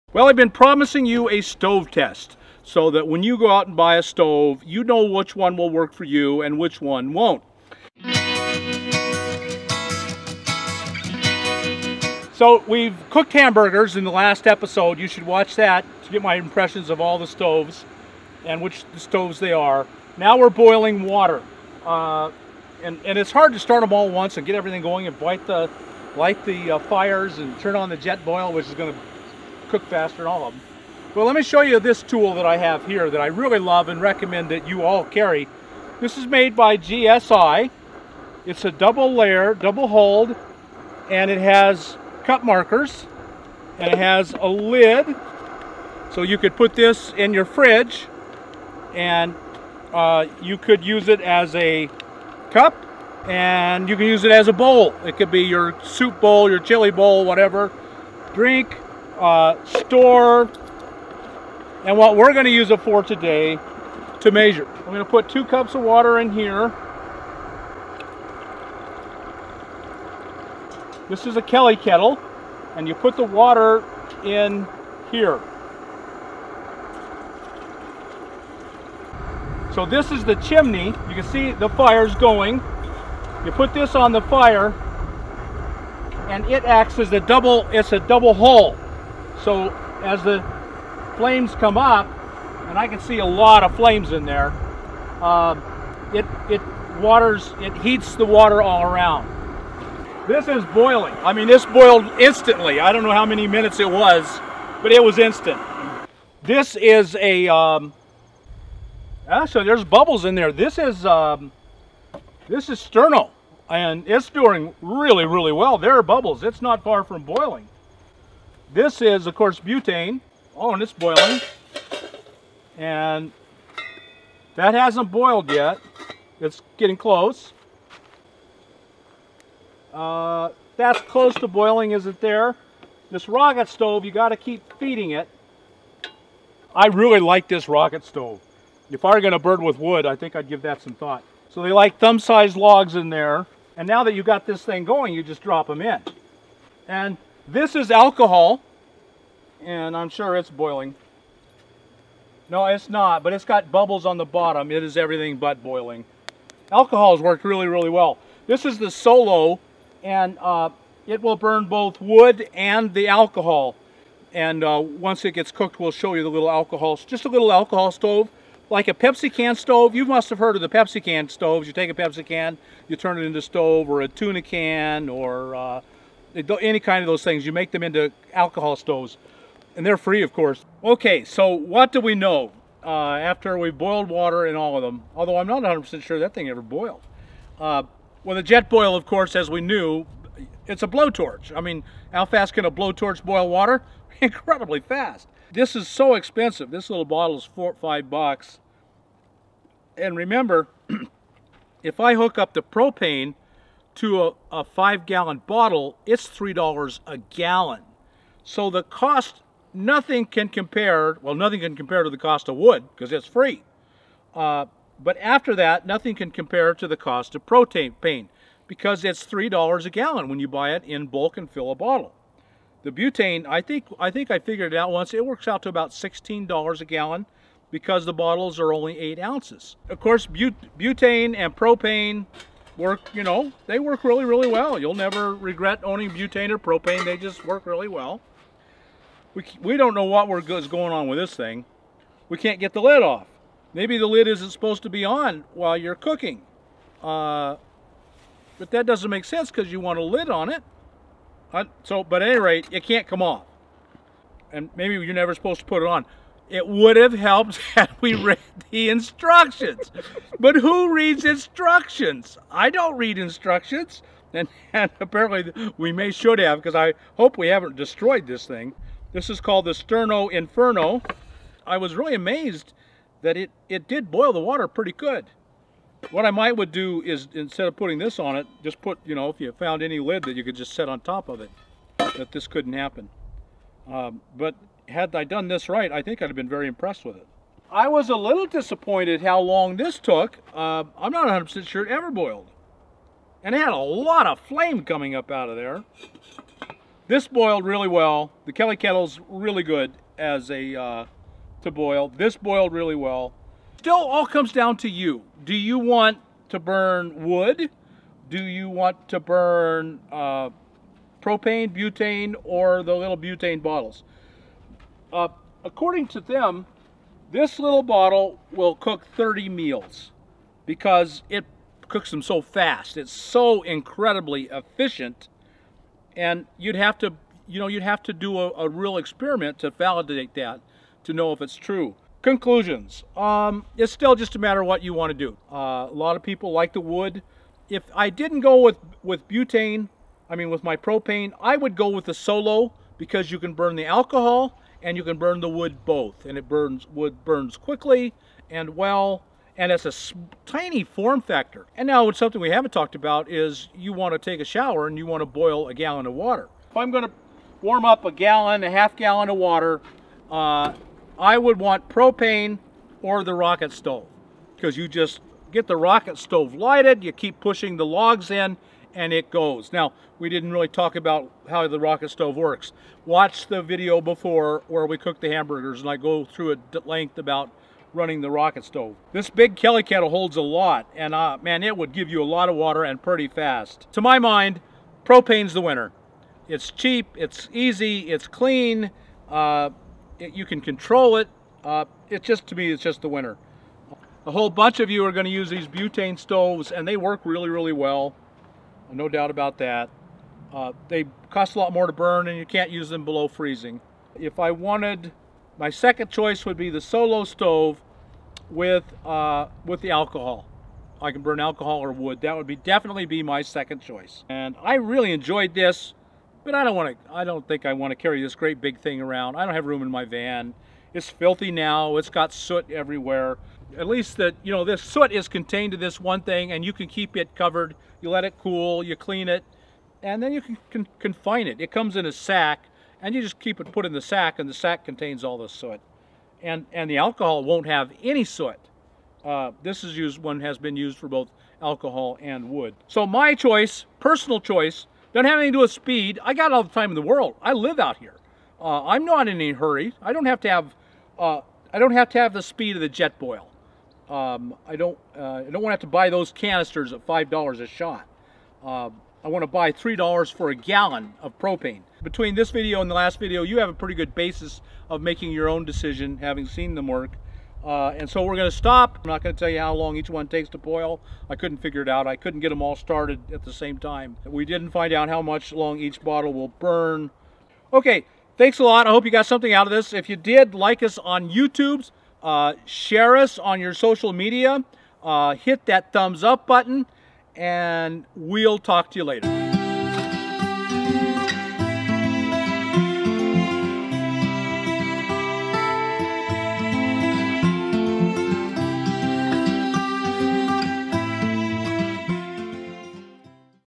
Stove Comparison and Review: Boiling Water with Butane, Propane, Alcohol and Wood CONCLUSION
In todays video I continue my comparison and test of 8 different stoves by Boiling water on each stove and reach my conclusions about their practicality after frying a hamburger and boiling water with each one of them.

*AMAZON LINKS - Items I Recommend:*
~Power Stations
~12V Batteries
~Portable Solar Panels
~Hard Solar Panels
~Gas and Dual Fuel Generators
~Air Conditioner Units
~Pop Up Tents